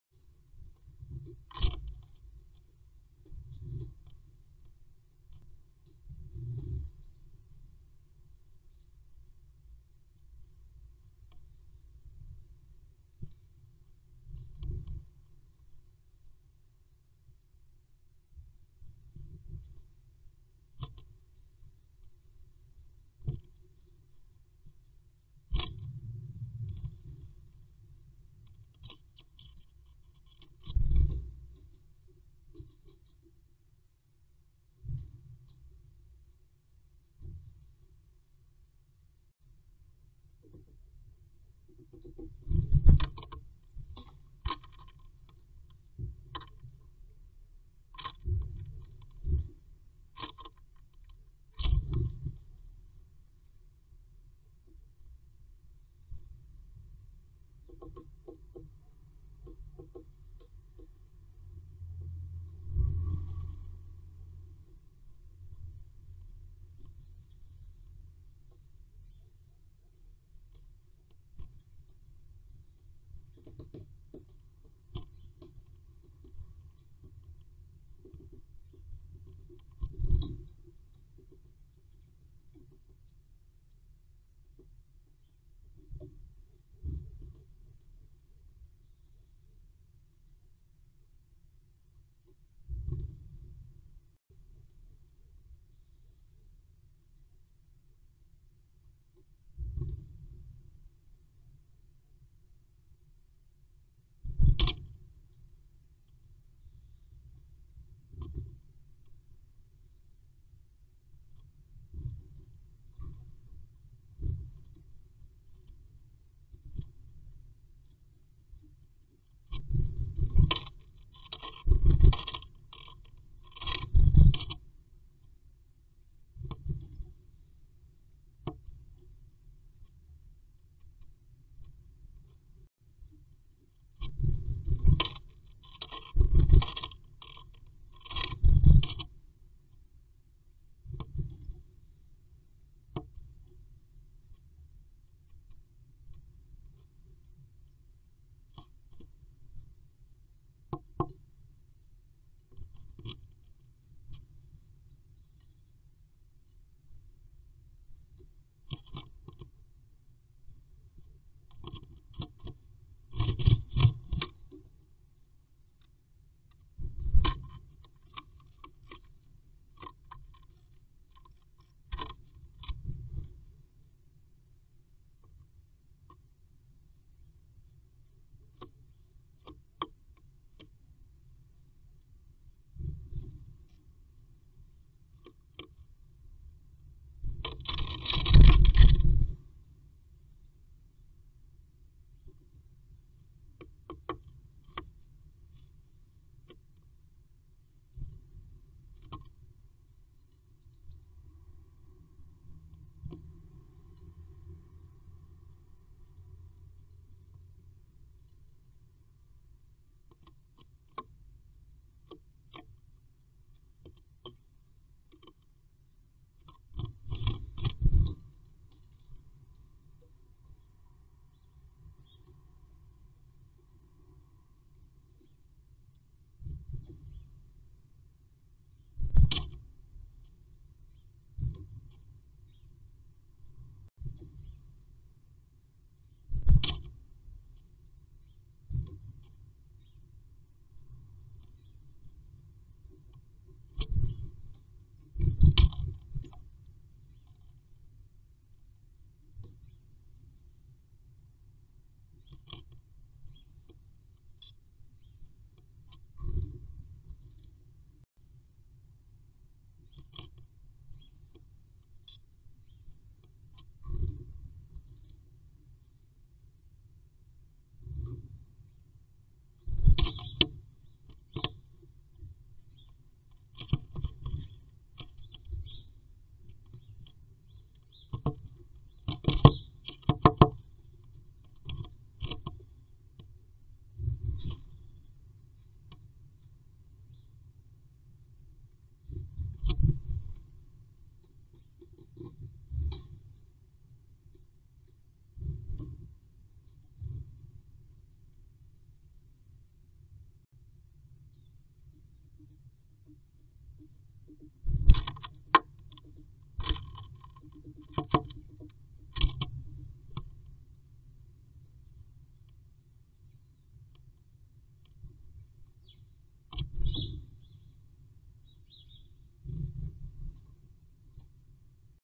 Zeitiger Besuch im Nistkasten/Great tit visits nest box early in the morning
Heute, ganz zeitig am Morgen, besuchte eine Kohlmeise  den Nistkasten und schaute sich ganz Neugierig um. Wird sie vielleicht bald über Nacht bleiben?